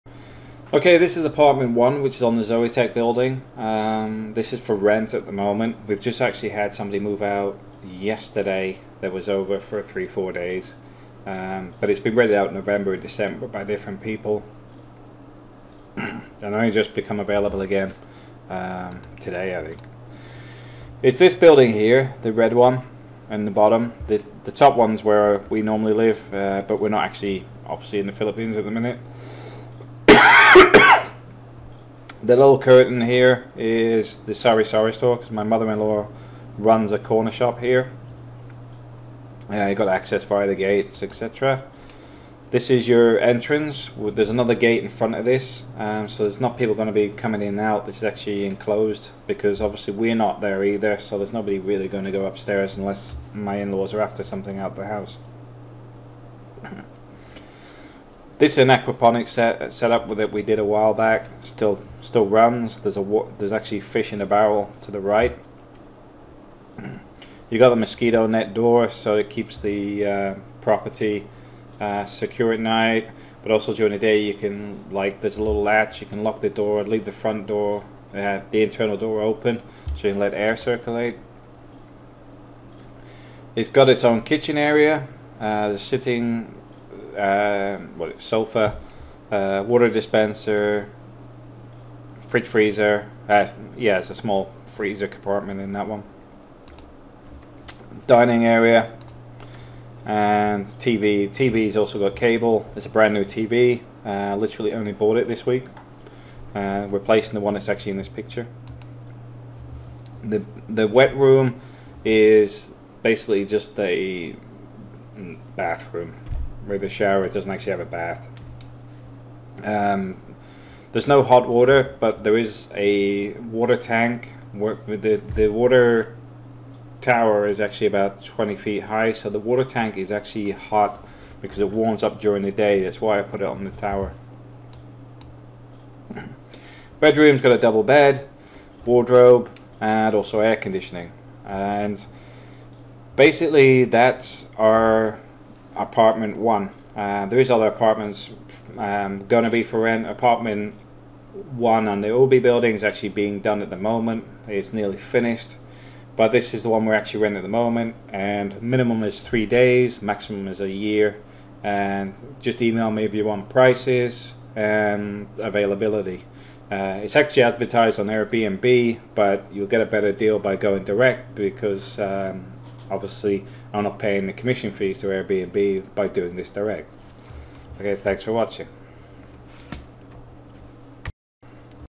Apartment for rent Minglanilla, Cebu, Philippines - P8,000 per month (+ Bills)
We are renting this apartment on a minimum of 3 days to a year. Its 5 minutes walk to the main road and AA BBQ, has Wi-Fi and TV. Air conditioning and secured by 24/7 CCTV coverage as well as being within a family compound.
*PRICE INCLUDES USE OF INTERNET, ELECTRIC, WATER (Short Term rentals only)

Several people have stopped in these apartments previously long-term and if you want more information on them please speak to Kento Ikeda , Tim Rodgers , Robert Dayton for a review.

1 bedroom with :-

Air conditioning
Fridge
Kitchen
Water dispenser
Private toilet and wet room facilities
Wi-Fi
TV
Cable TV
Own entrance

Pricing -

1 Night - P1,000
3 Nights - P2,100 (700 a night)
7 Nights - P4,200 (600 a night)
1 Month - P14,000
3 Month - P12.000 (per month) + 1 month deposit + first month + bills
12 Month - P8,000 (per month) + 2 month deposit + first month rent + bills

Sari-Sari store (Convenience store) is next door run by in-laws offering daily essentials such as shampoo, washing powder, soft drinks, beer. We do not offer credit at the store so please pay as you go as refusal can offend.

Rental Rules -
1 - 50% return of deposit if cancellation is 1 month notice prior to moving in.
2 - No rental is "guaranteed" unless deposit is paid in advance.
3 - For short term rental of upto 1 month payment should be made upon arrival before receiving the keys.
4 - Apartment should be returned in the same condition you received it.
5 - Any maintenance issues please report as soon as possible so they can be rectified.
6 - 3+ month rentals require to give 1 months advance notice to allow readvertising the apartment(s).
7 - Internet and Cable TV are a courtesy and not guaranteed to always be available due to Philippines infrastructure.
8 - All damages will be paid for.
9 - You are responsible for the safety and security of your belongings. We do not take responsibility for any lost or theft of items.
10 - No animals or pets to be kept in the property or on the grounds of the property.
11 - No extra tenants above that which is agreed maximum occupancy.
12 - No smoking inside the property.